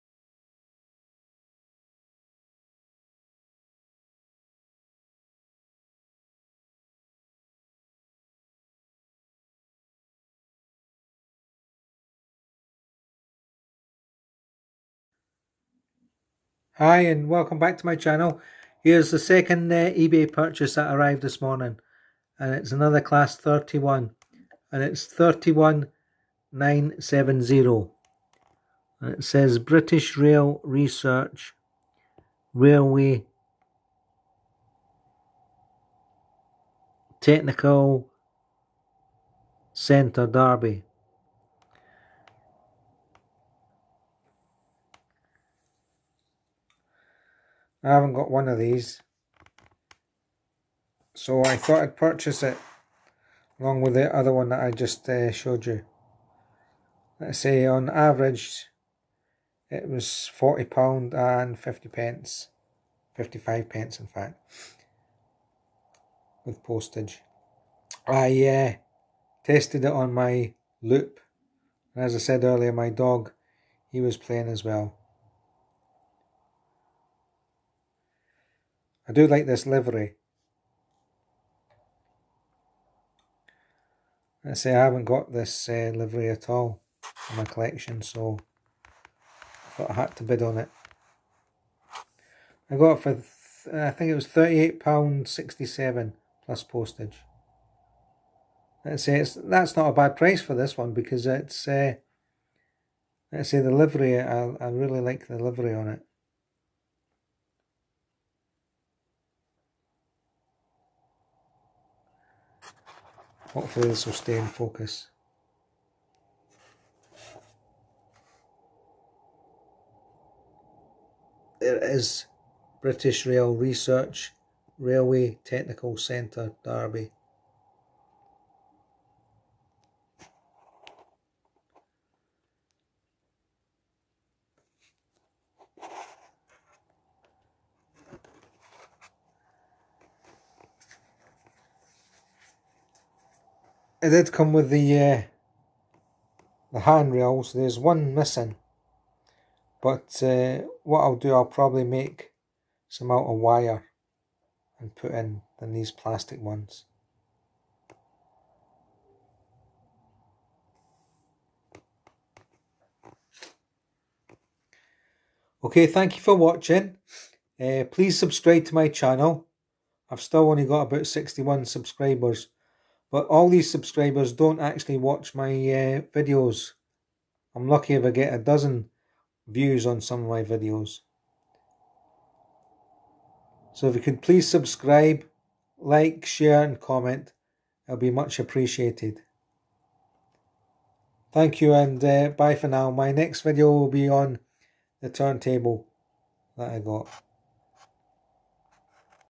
British Rail. Research Railway Technical Centre, Derby. Class 31970
This is just a quick video of Ebay purchase that arrived this morning. Lima Class 31 370 British Rail Research Railway Technical Centre, Derby £40.55 average with combined postage.